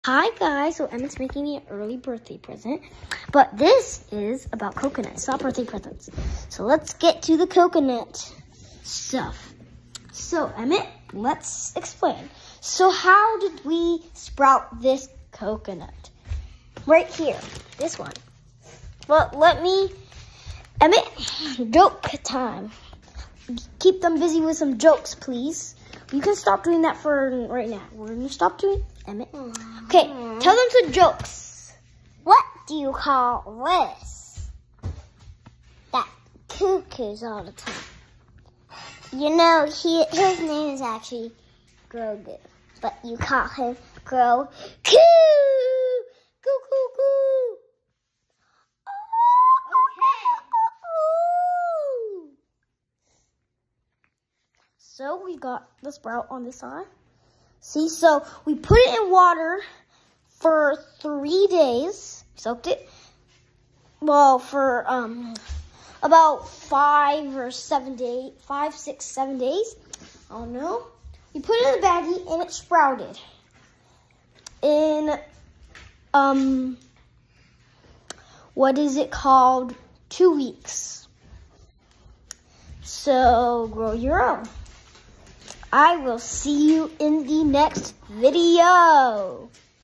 William and Emmett's Coconut Video
In this video these two crazy kids will teach you how to sprout a coconut